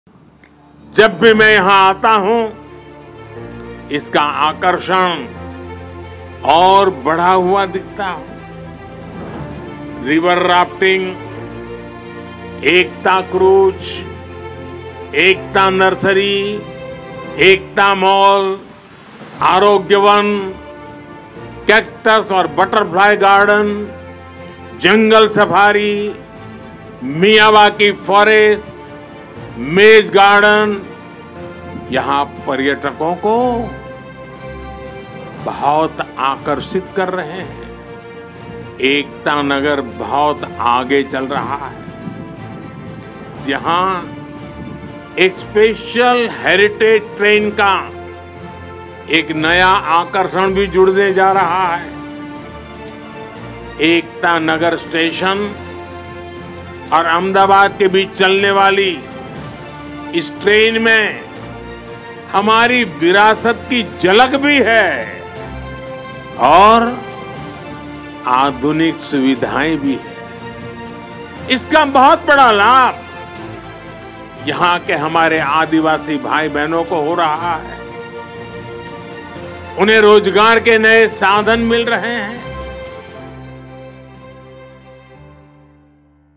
PM Narendra Modi Highlights The Beauty Of Attractions In Ekta Nagar
PM Narendra Modi participates in the Rashtriya Ekta Divas event in Ekta Nagar. highlights the beauty of the various attractions of Ekta Nagar.